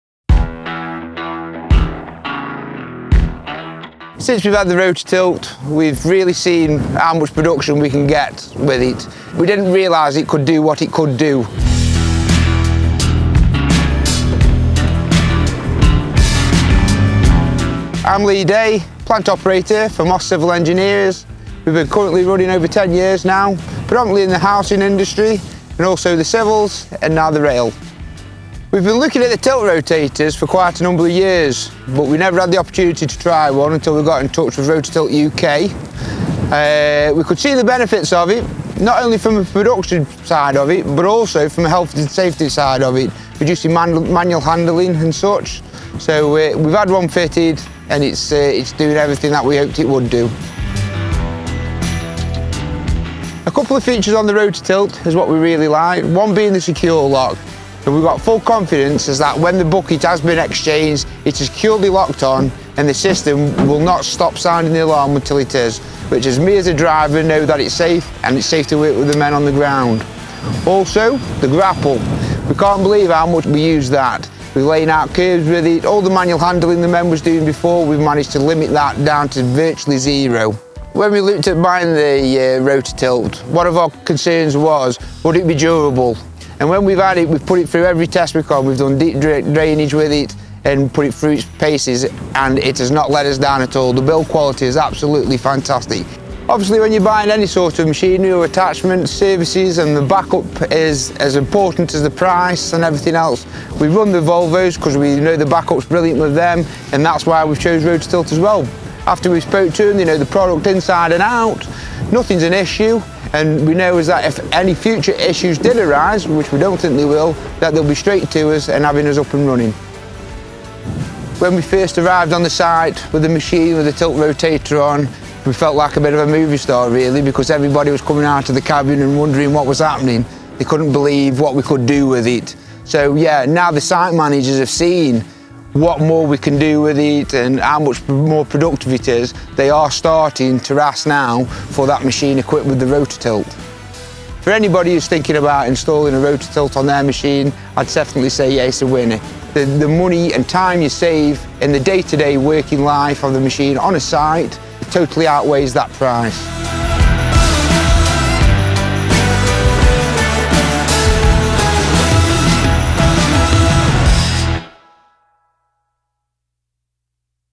Rototilt - Customer Lee Day shares his thoughts about Rototilt
"Since we've had the Rototilt we've really seen how much production we can get with it. We didn't realize it could do what it could do. We could see the benefits of it, not only from the production side of it, but also from a health and safety side of it, reducing manual handling and such. Now the site managers have seen what more we can do with it, and how much more productive it is, they are starting to ask for that machine equipped with the Rototilt."